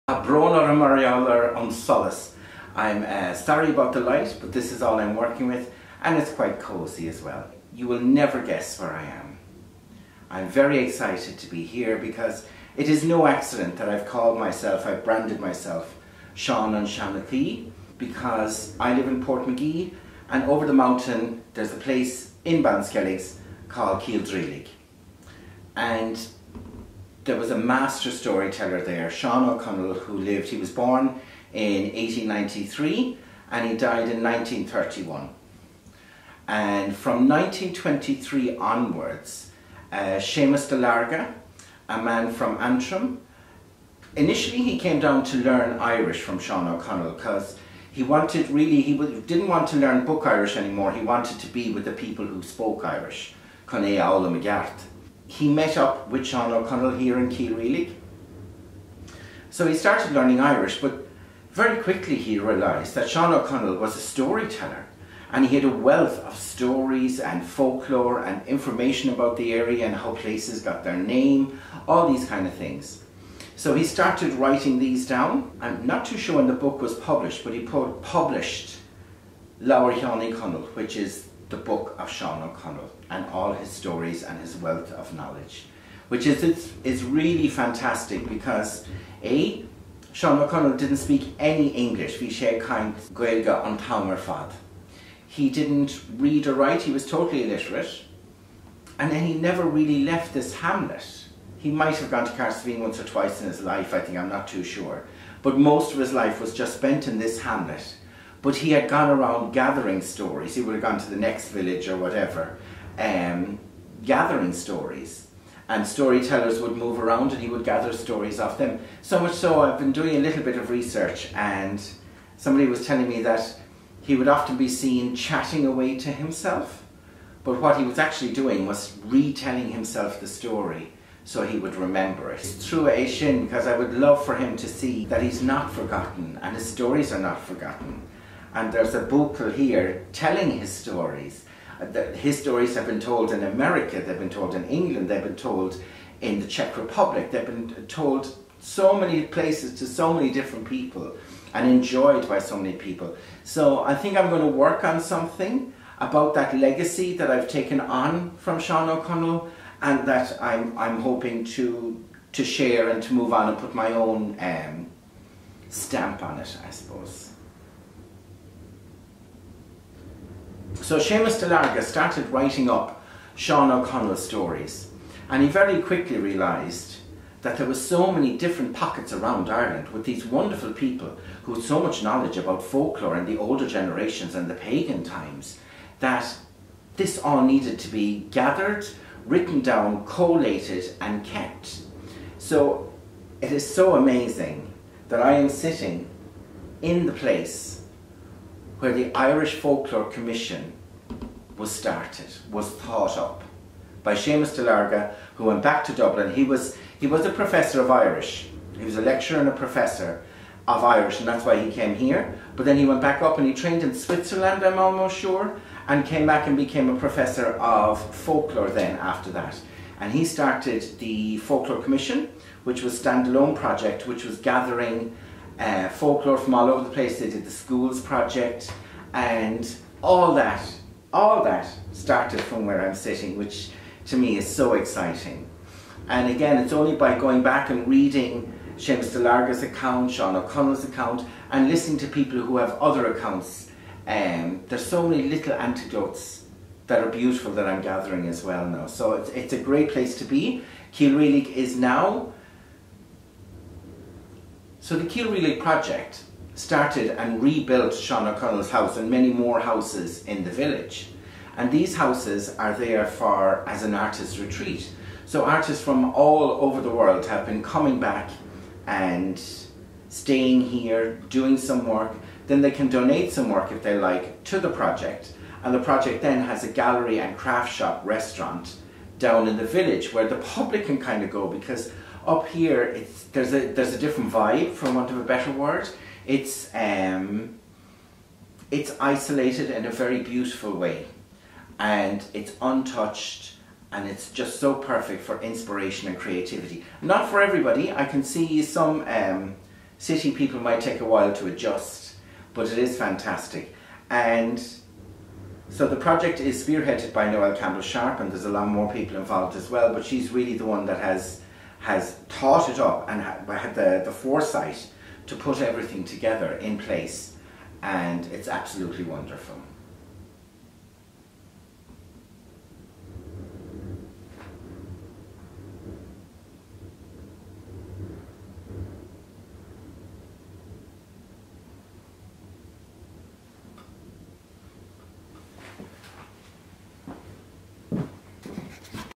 A blast from the past, last year at Cill Rialaig, Seán an Seanchaí, Ireland, Storyteller.
Last year, while on residency at the Cill Rialaig Project.
I made this video and just found it.  It's good to get things out like this, rather than leaving them in limbo?
Again, sorry for lack of light...